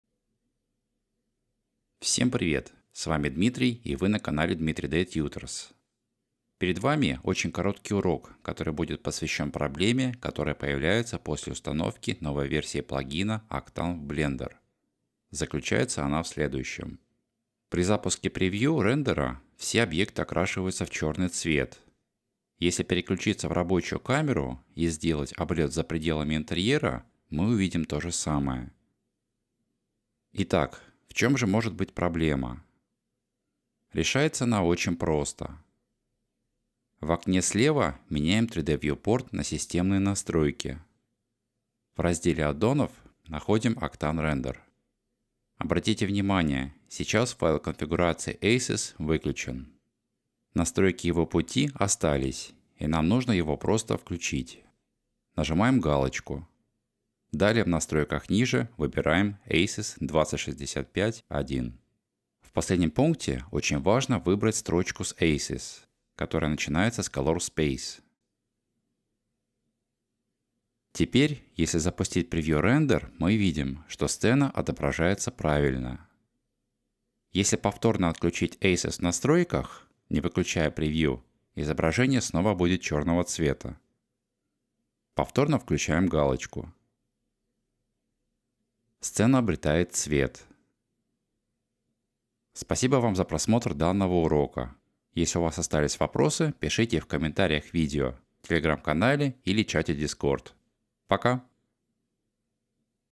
Blender Octane ACES проблема с отображением
Перед вами очень короткий урок, посвященный проблеме, которая появляется после установки новой версии плагина Octane в Blender.